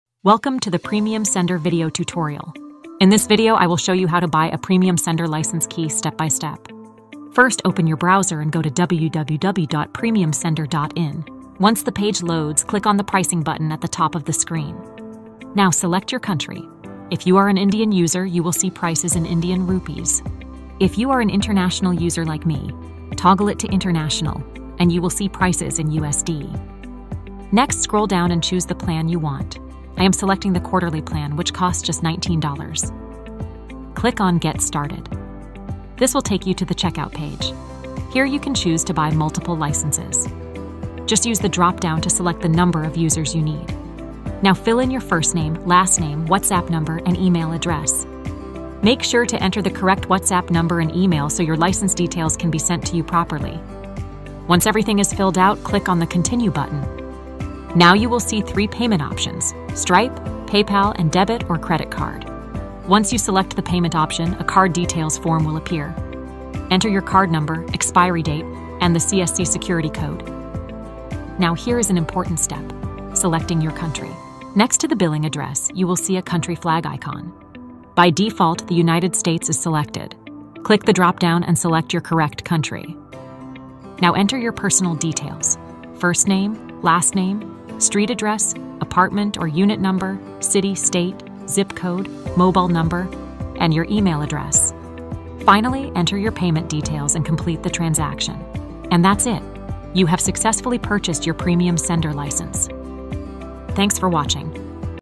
2. How to Buy Premium Sender | Step-by-Step Guide
Premium Sender Extension - The Ultimate Messaging Tool 🚀

Premium Sender is a powerful Google Chrome extension designed to enhance your messaging experience. Whether you need to download contacts, send bulk messages, or manage your communication efficiently, Premium Sender has got you covered!

🔹 Key Features:
✅ Download contacts from chats & groups
✅ Extract saved, unsaved, and labeled contacts
✅ Send bulk messages with personalization
✅ Automate and schedule messages
✅ Easy-to-use interface with safe and secure processing

💡 How to Use Premium Sender?
1️⃣ Install the Premium Sender Extension from the Chrome Web Store.
2️⃣ Log in to your web messaging platform.
3️⃣ Use the extension to download contacts or send messages in bulk.
4️⃣ Enjoy a seamless messaging experience!

💬 Need Help? Drop your queries in the comments or contact support.